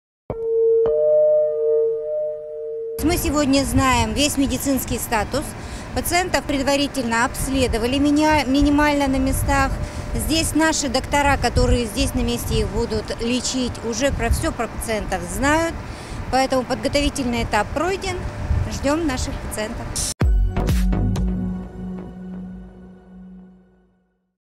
Тюменские медики в Краснодоне провели телемедицинские консультации
"Тюменские медики в Краснодоне провели телемедицинские консультации, пациентов предварительно обследовали. Доктора, которые будут лечить этих людей в Тюмени, уже все про них знают", - сказала директор департамента здравоохранения Тюменской области Наталья Логинова.